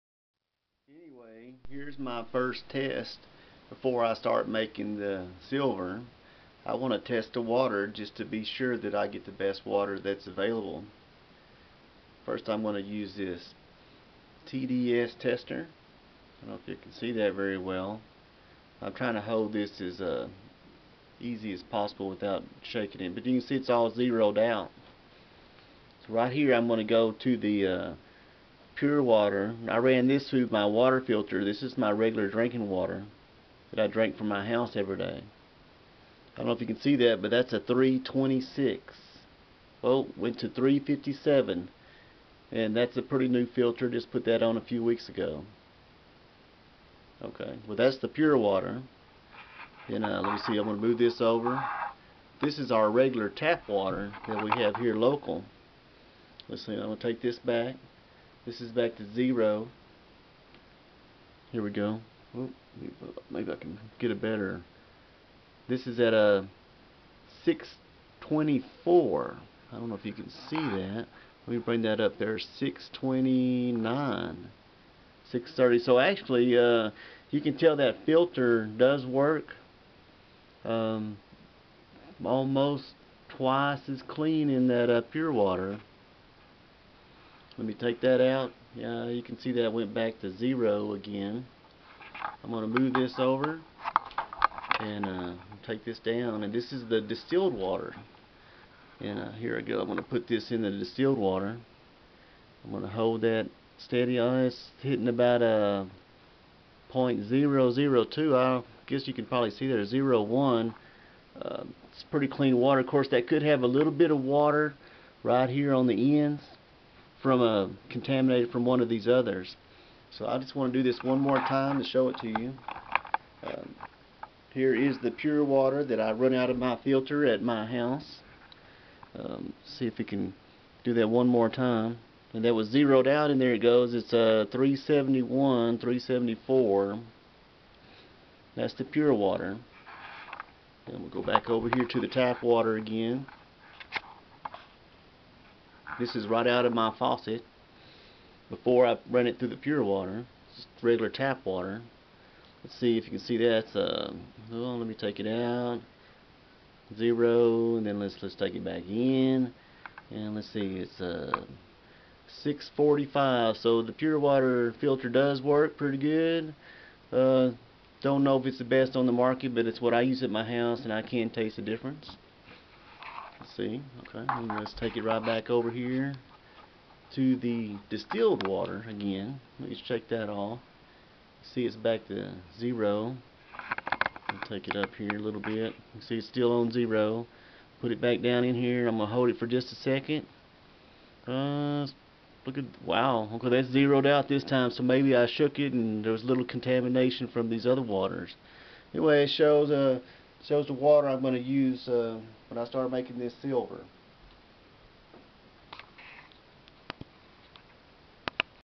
tap water pur distilled test ppm for making silver colloidal project
Here is a simple ppm water test to help you decide which water is the best when drinking or using to make silver colloidal. The water with the most impurities is compared from regular tap water, using the pure filter and distilled water.  The higher the pps parts per million number, then the more stuff in your water.  Did you know that you can contact your local water supplier and they must tell you what is in your water.  It must pass certain conditions to be legally drinkable. Of course some of the items in the water are good for you, for example minerals. Yes, I know that I miss-spelled distilled water - was in a hurry :)

If you liked my video about colloidal silver testing please comment.